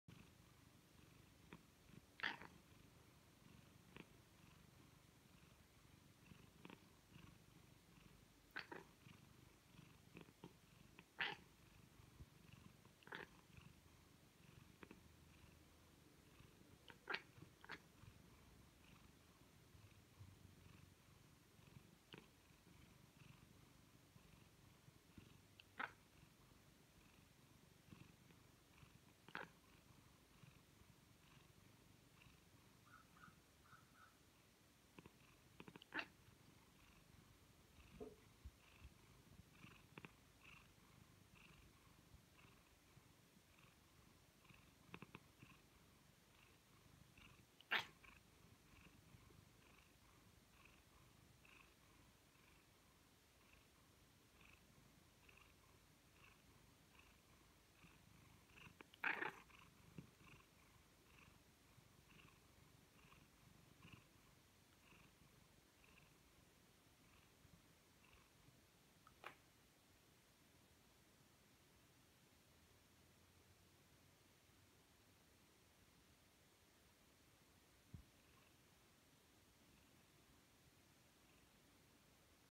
Lola Purring and Chattering
Lola sees a bird out the window while perched on the arm of my office chair.